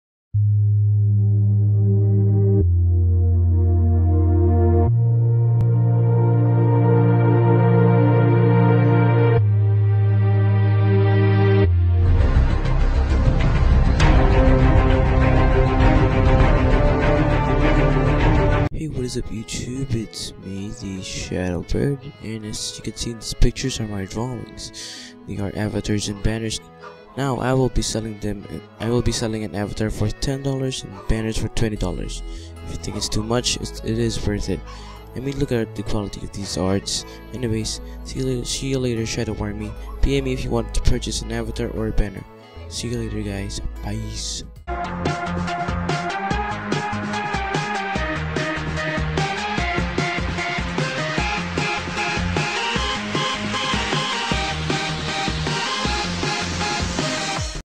SELLING AVATARS AND BANNERS
Hey shadow army, i will be announcing again that i will be selling avatars and banner, avatar for $10 and $20, i know it's too much but its so worth it. in this video i think we can go to 2-5 likes dont forget to like and subribe enjoy!

*MUSIC CREDITS TO OWNERS*

Cya later shadow army
STAY ROCKIN!